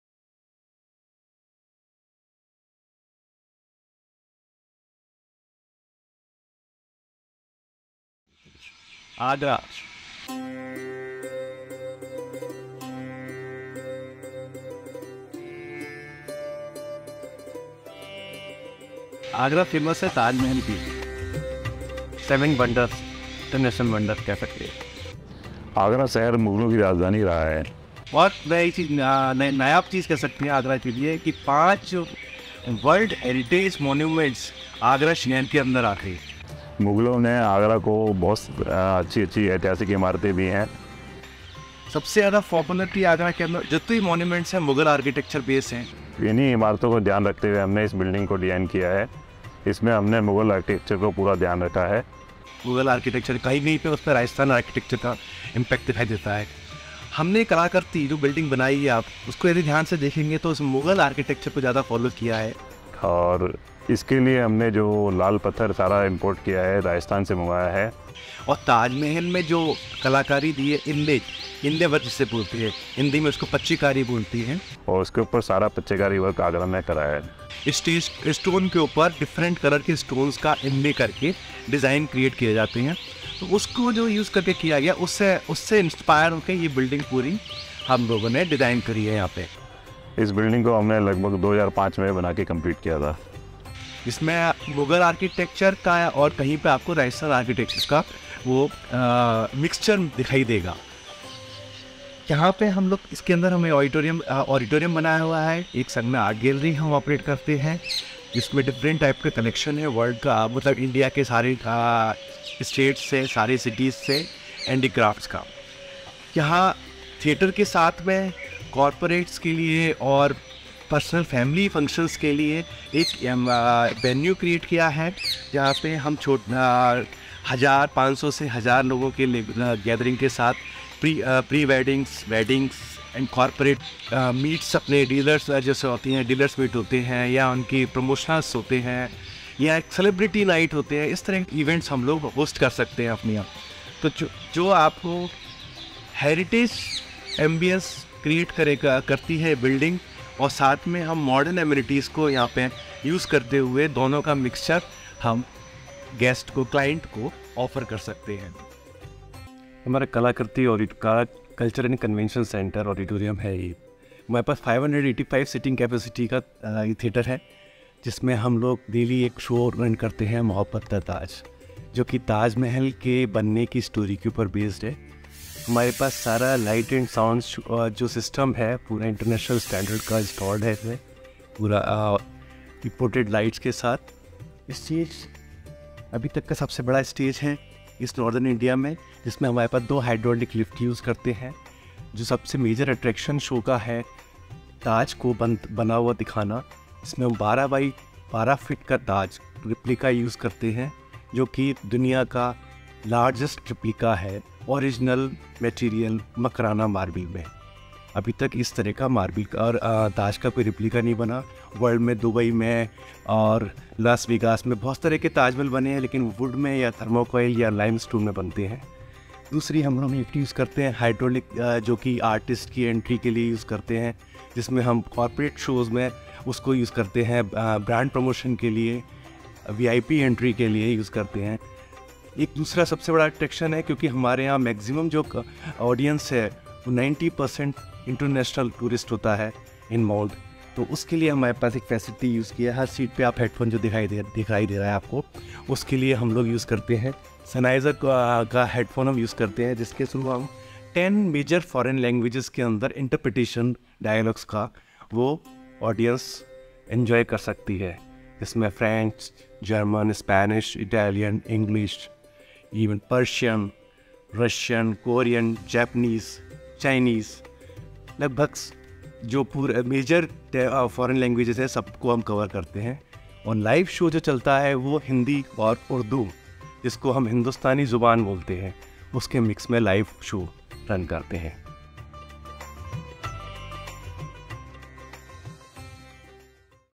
EP 14 - S1 | EXPLORING MOHOBBAT THE TAJ | KALAKRITI
Welcome to the first video of "Exploring Agra beyond Taj".

Special Thanks to Shri Ashok Kr. Jain | Chairman: Ashok Oswal Group & Kalakriti.

The Kalakriti Cultural and Conventional Centre is an arts, culture and conventional centre in Agra. The aim of the centre is to celebrate the beauty and uniqueness of Indian culture and heritage. The centre offers visitors a chance to view intricate inlay work on marble.

Apart from that, the Kalakriti Cultural and Convention Centre has a handicrafts showroom where one can purchase items made of marble, metal, wood and thread. The key attraction of the Kalakriti Cultural and Convention Centre remains the popular musical drama, Mohabbat-the-Taj: The Saga of Love, which is a series of plays staged every day at 6:30 pm.